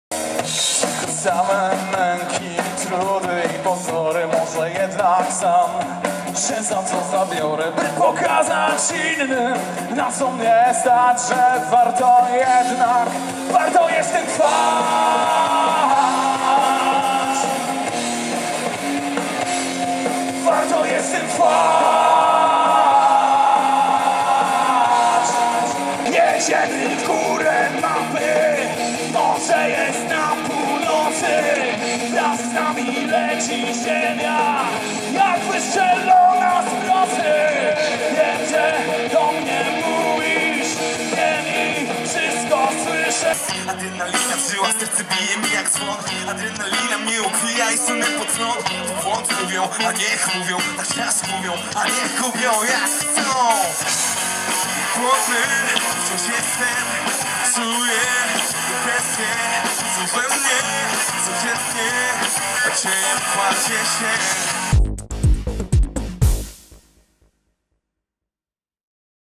Piastonalia 2014. Wtorek - koncerty
Piastonalia 2014 - koncerty przy Studenckim Centrum Kultury oraz na błoniach Politechniki Opolskiej.  Więcej informacji z regionu na